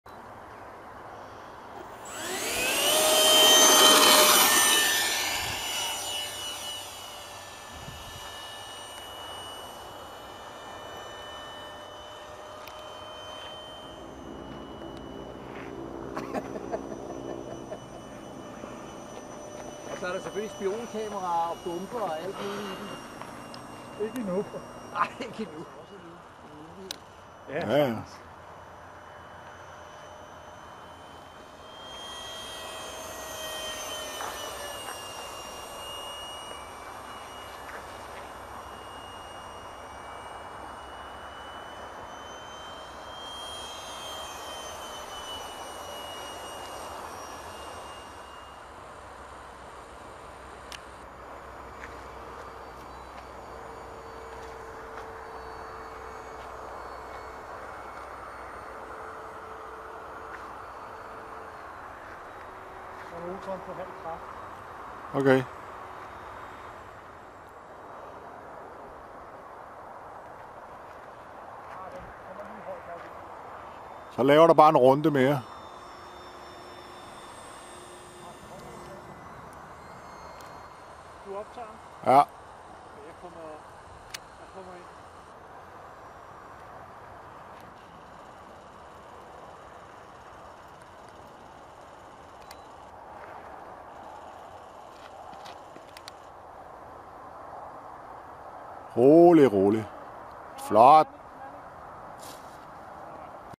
F-18 Blue Angels from Art-Tech
This is my very first EDF Jet. The F-18 Blue Angels from Art-Tech. Every thing, so far, is stock. The only mod done, is cutting a new Lipo hatch behind the canopy, to fit a 3s 2200mAH Lipo.